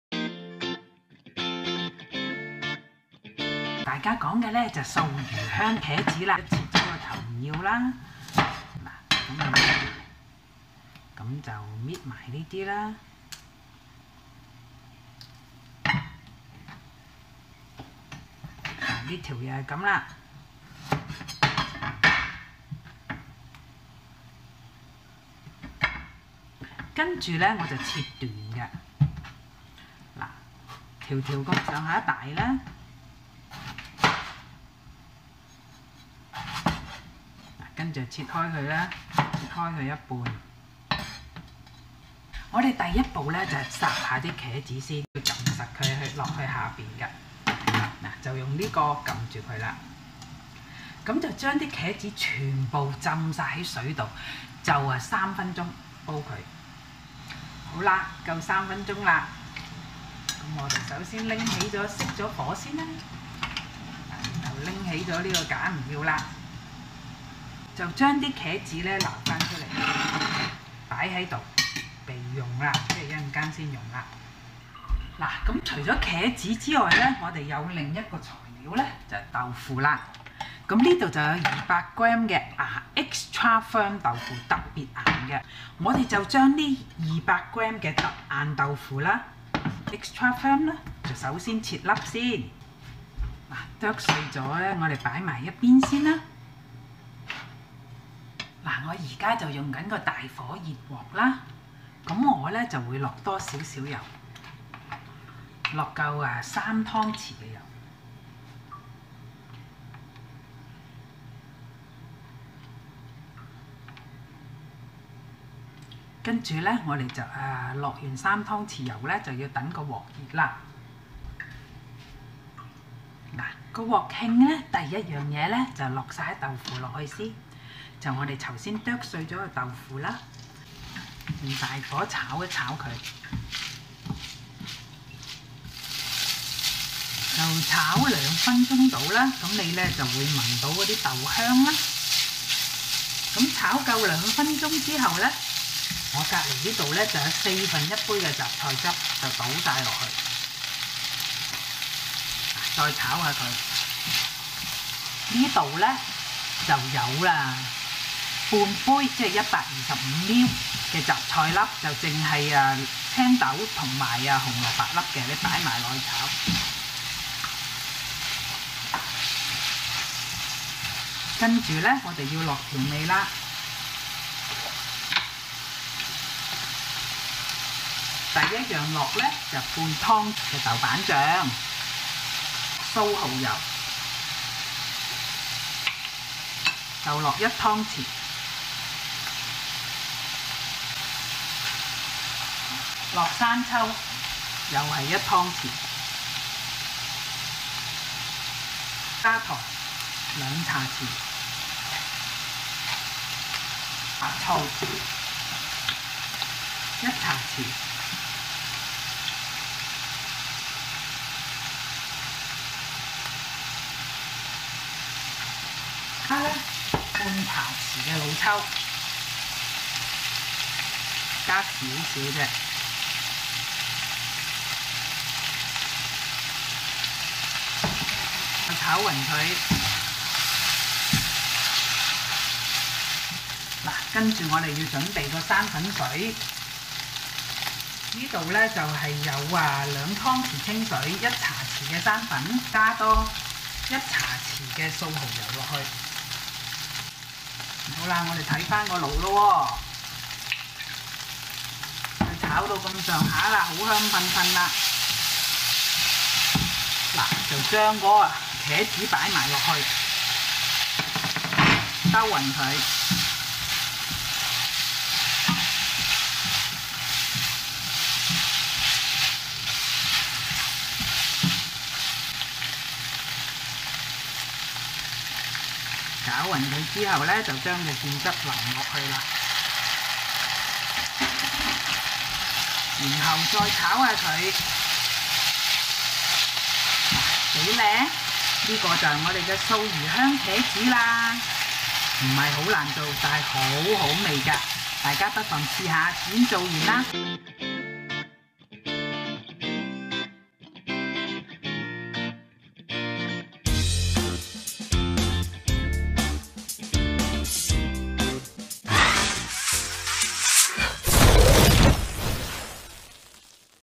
素魚香茄子 Vegan Sichuan Eggplant Stirfry 🍆
材料ingredients:
300g 中國茄子 Chinese eggplant
200g 硬豆腐 extra firm tofu
1/2 cup (125ml) 紅蘿蔔和青豆粒 diced carrot n green peas
1/4 cup (63ml)雜菜汁 8 veggies juice

調味seasonings:
1/2 tbs 豆瓜醬 Toban chili sauce
1 tbs 素蠔油 vegan oyster sauce
1 tbs 生抽(豉油) soy sauce
2 tsp 糖 sugar
1 tsp 白醋 white vinegar
1/2 tsp 老抽 dark soy sauce

生粉水 starchy solution
拌勻mix
2 tbs 水 water
1 tsp 生粉 potato starch
1 tsp 素蠔油 vegan oyster sauce

做法cooking:
1.先把300g茄子去頭切段
cut off the eggplant stalks, cut them in length, then in half
2. 將茄子放在滚水中
put eggplant in boiling water
3. 用架壓在水中大火煲3分鐘
use a rack to keep them submerged in water n boil for 3 mins
4. 撈出備用
scoop them out for later use
5. 把200g硬豆腐切粒再剁碎
mince the tofu
6. 大火熱鑊下3湯匙油
set pan on high heat with 3 tbs oil
7. 下碎豆腐大火炒炒約2分鐘
fry the minced tofu on high heat for 2 mins
8.下雜菜汁,炒至豆腐吸乾菜汁約1分鐘
add veggies juice n fry till tofu absorb all the juice in about 1 min
9. 下紅蘿蔔和青豆粒炒炒
add carrot n green peas n give it a stir
10. 下調味炒勻
add seasonings n stir it
11. 下茄子炒炒
add eggplant n stir it
12. 下生粉水炒炒即成
add starchy solution, stir it then done